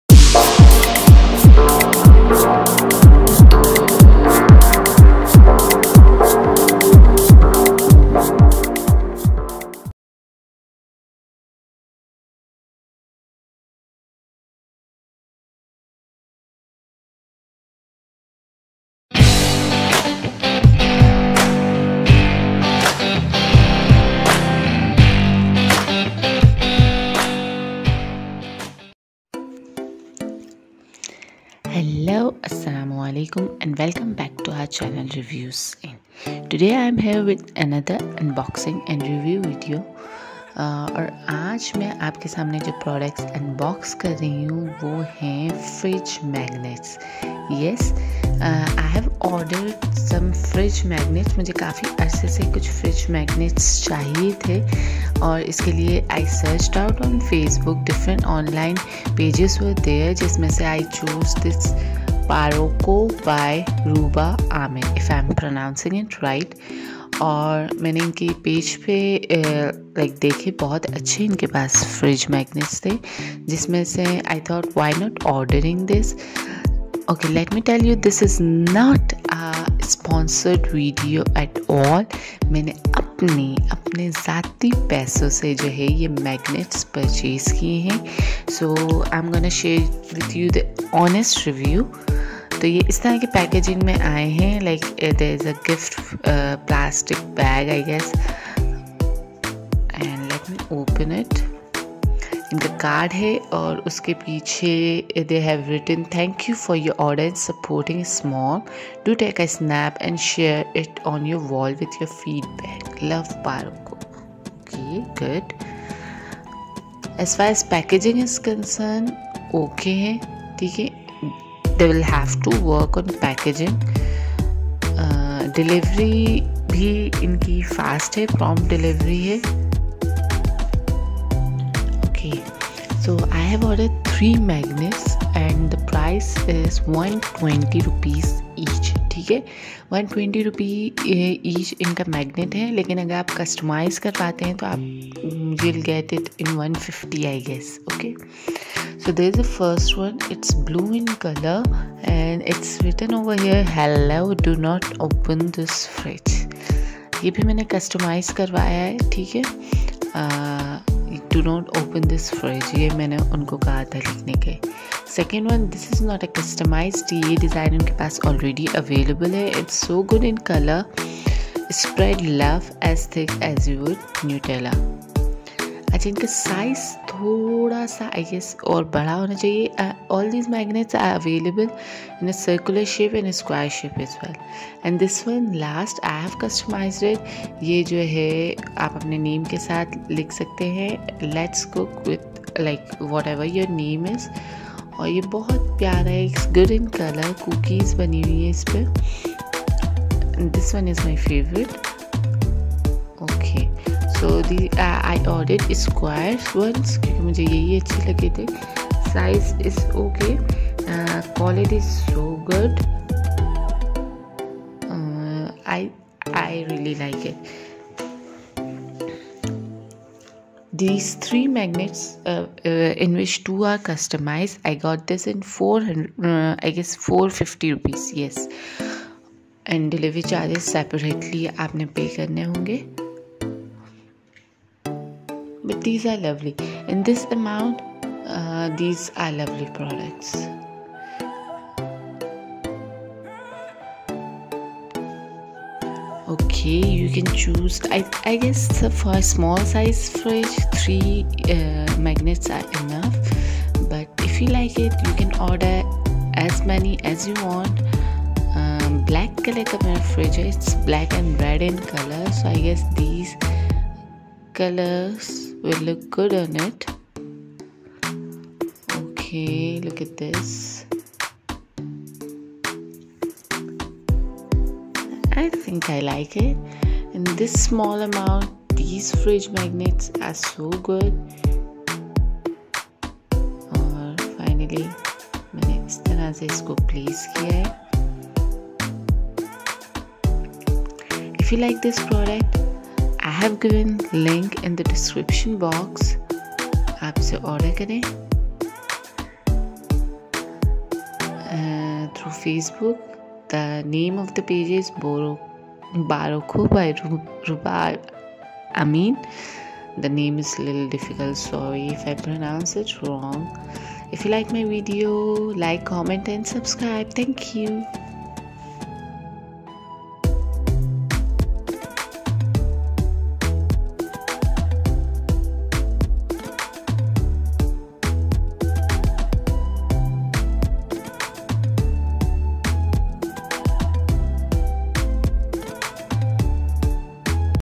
Fridge Magnets | Unboxing and Reviewing | Reviews Inn
In today's vlog I am gonna do unboxing of some fridge Magnets that I have ordered online by